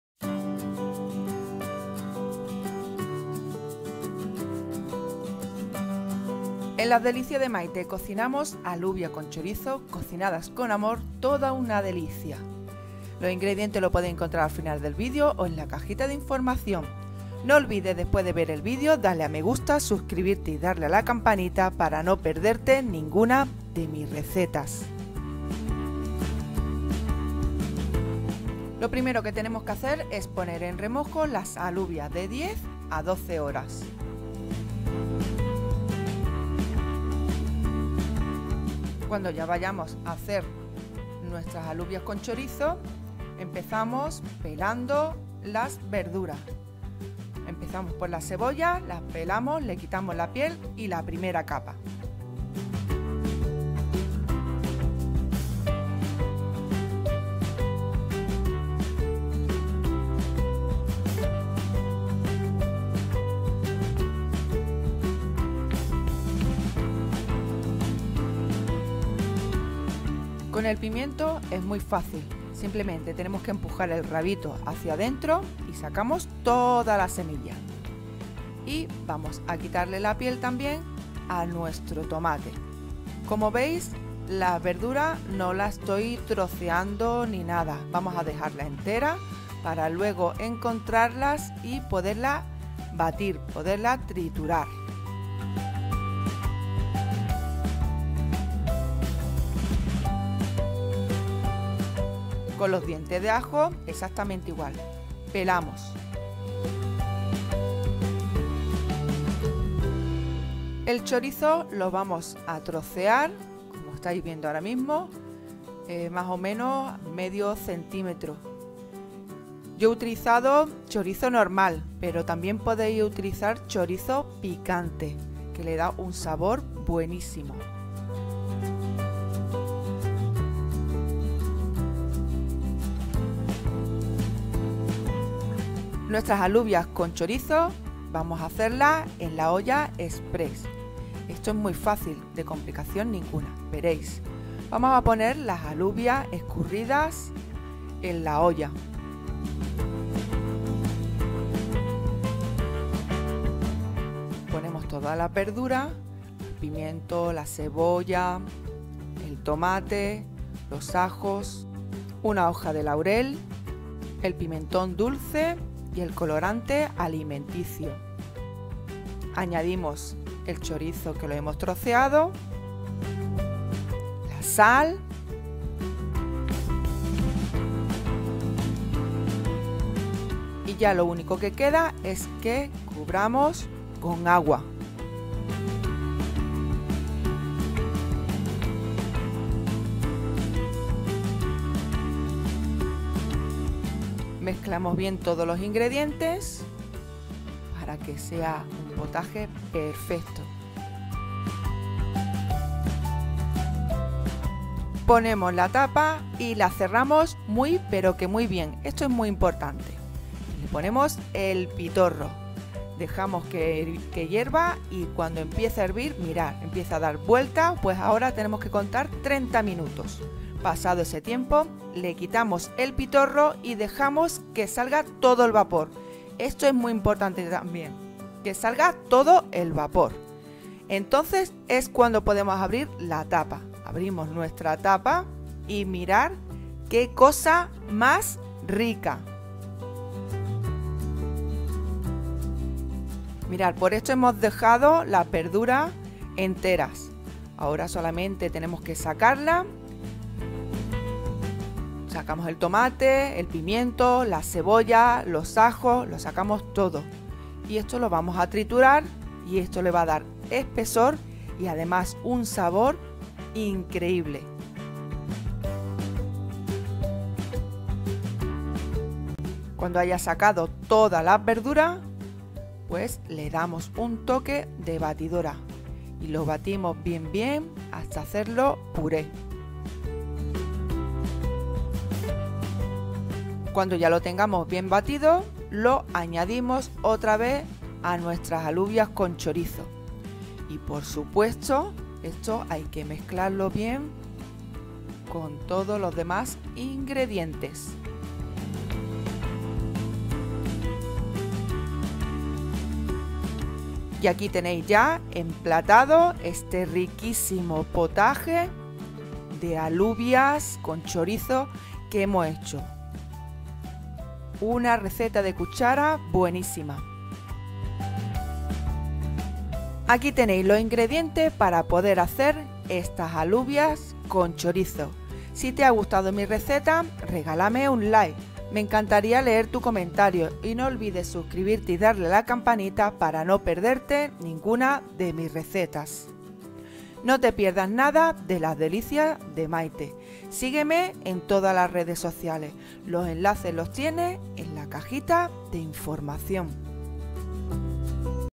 Alubias con chorizo, cocinadas con amor es toda una delicia
Alubias con chorizo, cocinadas con amor es toda una delicia, receta de cuchara para calentar el cuerpo.

INGREDIENTES:
- 400gr. de alubias blancas.
- Un chorizo.
- Un tomate.
- Un pimiento verde.
- Una cebolla.
- 3 dientes de ajo.
- Una cucharadita de pimentón dulce.
- 1/2 cucharadita de colorante alimenticio.
- Una hoja de laurel.
- Sal.

INGREDIENTS:
- 400gr. of white beans.
- One chorizo.
- One tomato.
- One green bell pepper.
- One onion.
- 3 cloves of garlic.
- A teaspoon of sweet paprika.
- 1/2 teaspoon of food coloring
- A leaf of laurel.
- Salt.